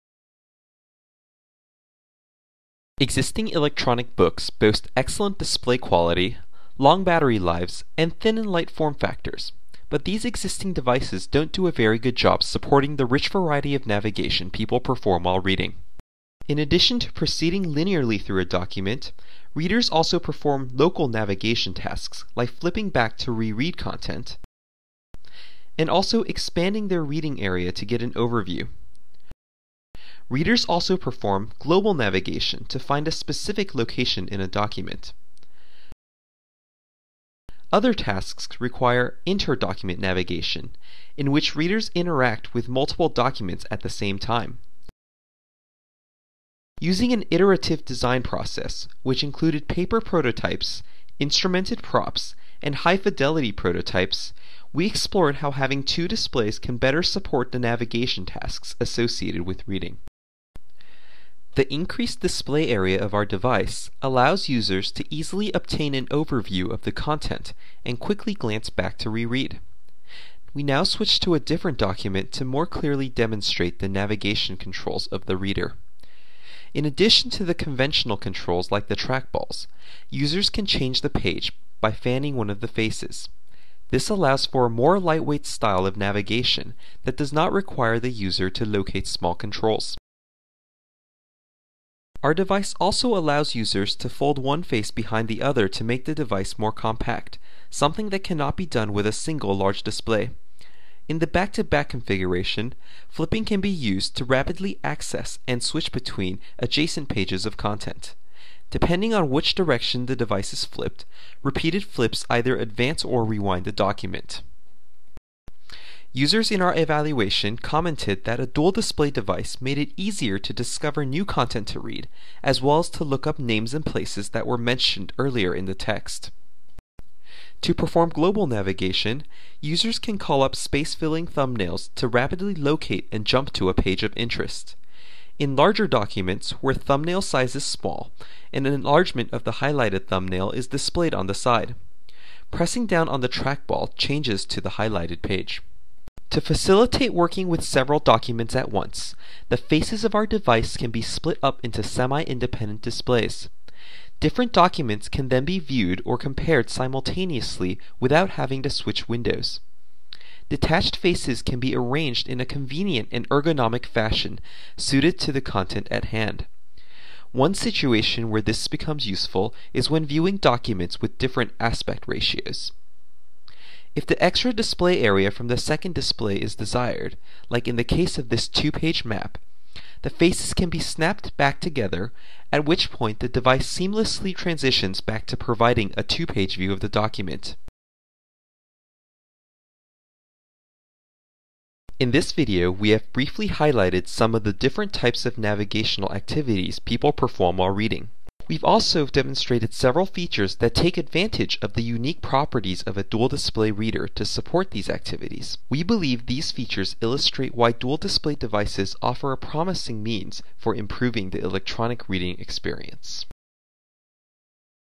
Navigation Techniques for Dual-Display E-Book Readers
Authors: Nicholas Chen, François Guimbretière, Morgan Dixon, Cassandra Lewis, Maneesh Agrawala

Abstract: Existing e-book readers do not do a good job supporting many reading tasks that people perform, as ethnographers report that when reading, people frequently read from multiple display surfaces. In this paper we present our design of a dual-display e-book reader and explore how it can be used to interact with electronic documents. Our design supports embodied interactions like folding, flipping, and fanning for local/lightweight navigation. We also show how mechanisms like Space Filling Thumbnails can use the increased display space to aid global navigation. Lastly, the detachable faces in our design can facilitate inter-document operations and flexible layout of documents in the workspace. Semi-directed interviews with seven users found that dual-displays have the potential to improve the reading experience by supporting several local navigation tasks better than a single display device. Users also identified many reading tasks for which the device would be valuable. Users did not find the embodied interface particularly useful when reading in our controlled lab setting, however.